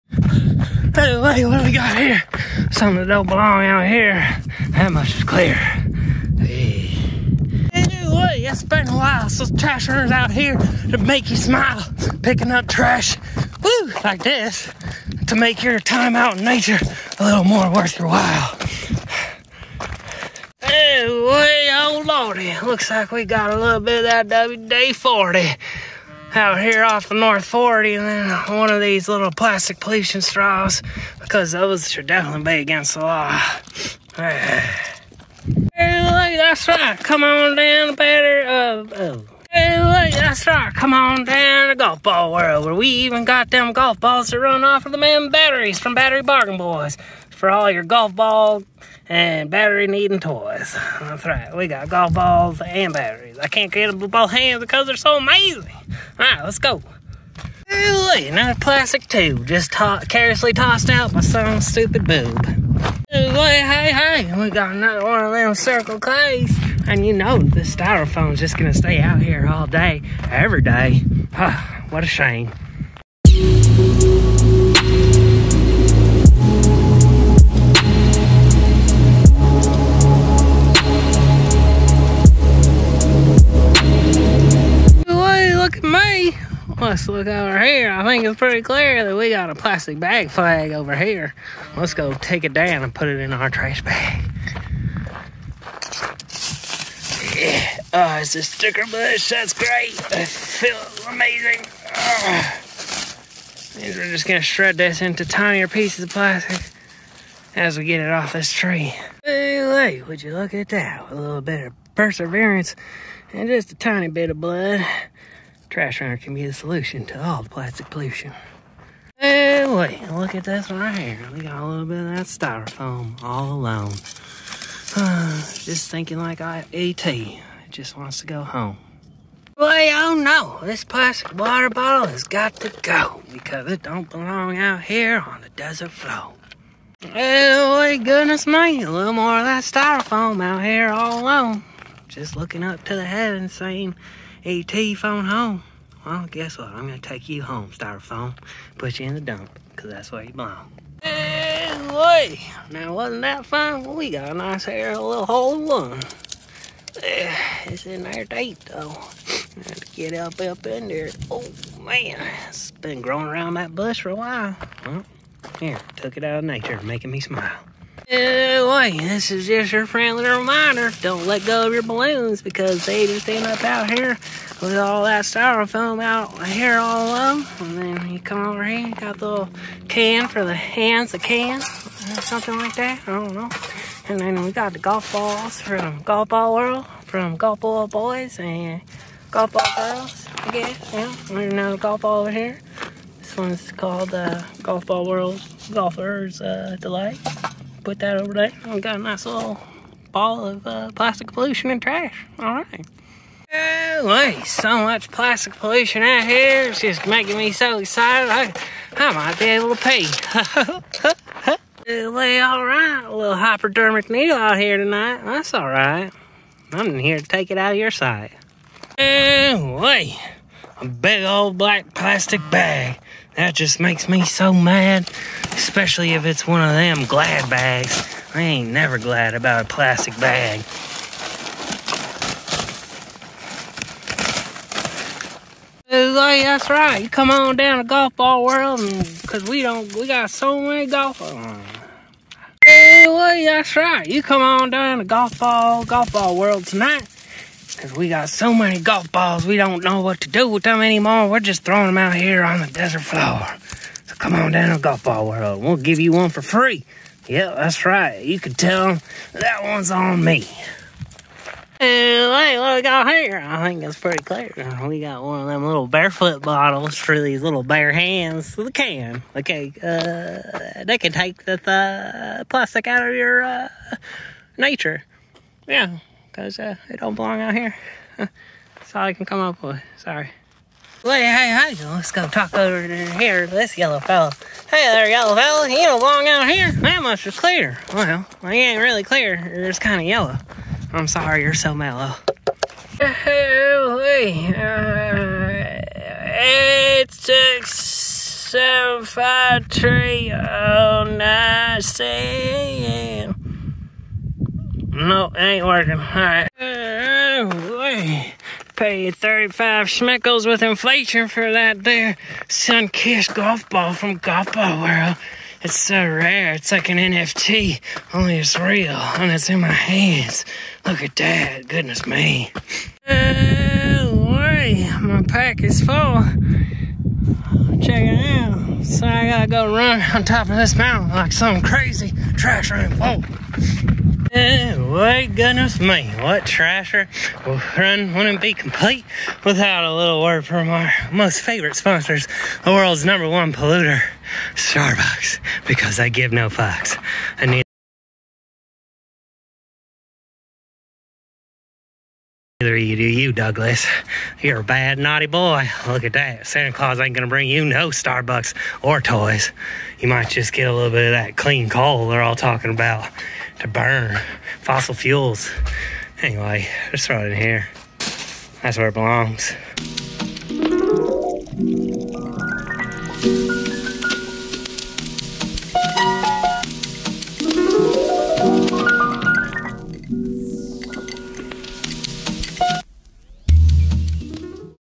Trashrunner episode 1⃣7⃣
Grateful for mother earth so I gotta run to keep her a lil' more clean! 🏃‍♀️ 🌎🏃🏿‍♂️🌍🏃🏽🌏 🚮